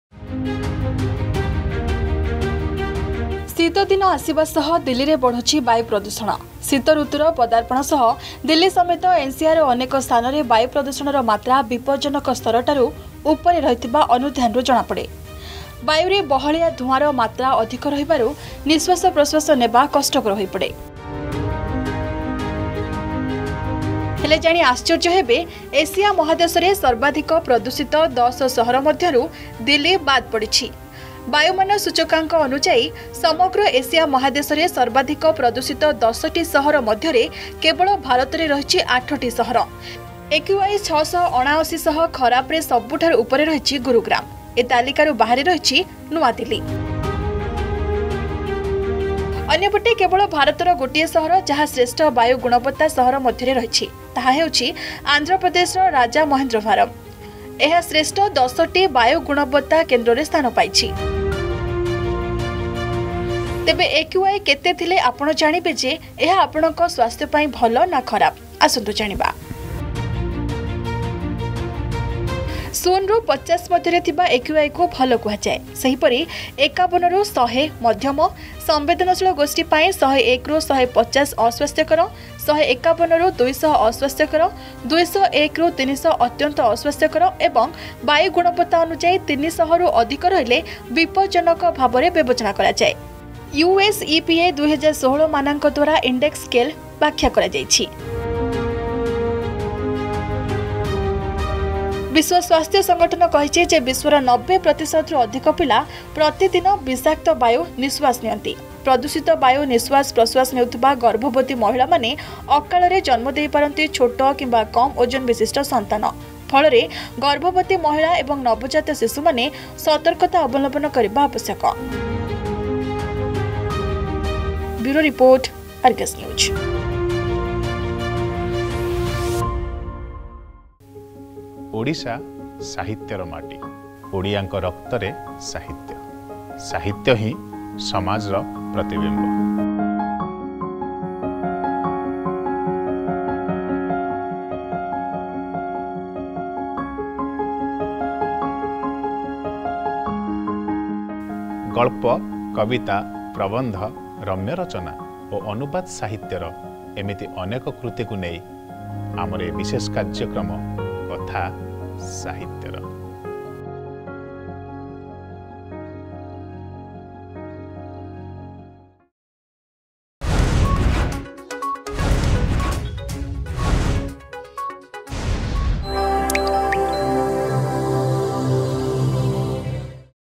8 Are From India Among 10 Most Polluted Cities In Asia, Delhi Not In List
8 Are From India Among 10 Most Polluted Cities In Asia, Delhi Is Not In List.